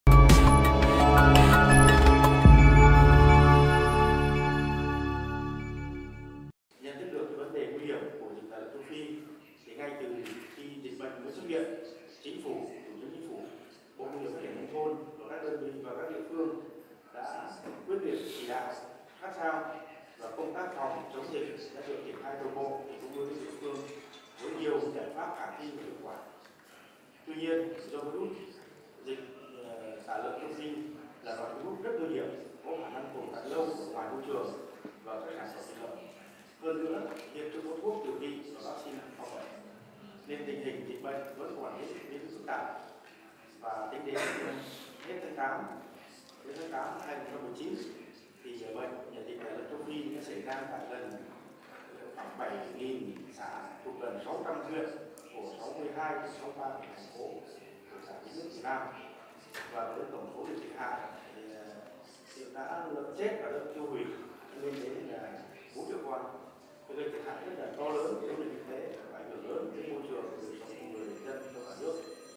Bệnh dịch tả lợn Châu Phi xảy ra tại gần 7000 xã
Ông Lê Quốc Doanh, Thứ trưởng Bộ Nông nghiệp và Phát triển nông thôn cho biết tại Hội nghị chia sẻ thông tin và kinh nghiệm "Phòng, chống bệnh dịch tả lợn Châu Phi" giữa Hà Lan và Việt Nam, ngày hôm nay 21/8.
Tính đến tháng 8/2019, bệnh dịch tả lợn Châu Phi đã xảy ra tại gần 7.000 xã thuộc gần 600 huyện của 62/63 tỉnh, thành phố khiến tổng số lợn phải tiêu hủy lên đến 4 triệu con.